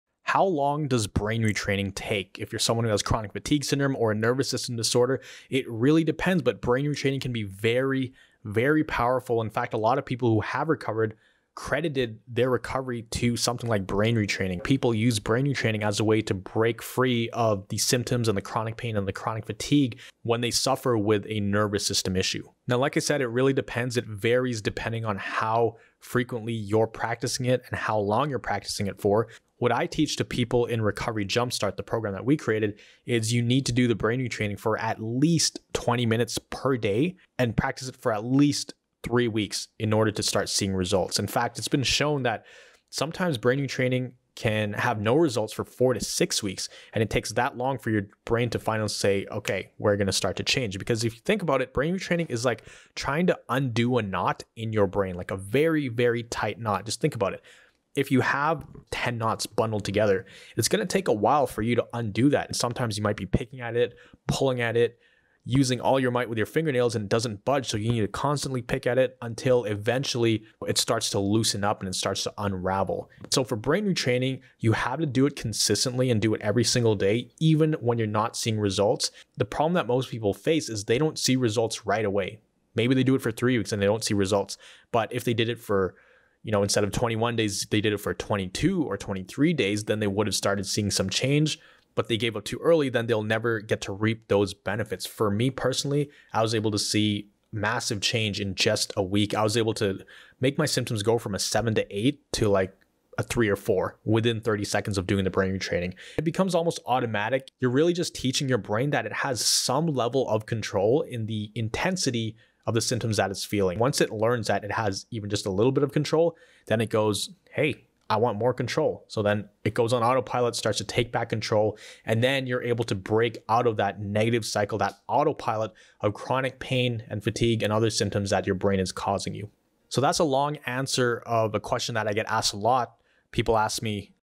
HOW LONG DOES BRAIN RETRAINING TAKE | CHRONIC FATIGUE SYNDROME
📈 See If Our System Can Solve Your Symptoms With This 3 Minute Quiz
You Are Just One Mindshift Away From Living Life With Thriving Health

Remember that when you do stuff outside your comfort zone and you do take those steps towards recovery, you will feel symptoms. There's not a single person. I know, including myself who didn't have symptoms during their recovery.

Your success and your recovery are determined by how well you respond to symptoms. So just know that symptoms are not the worst thing ever and don’t mean your recovery is failing or not working.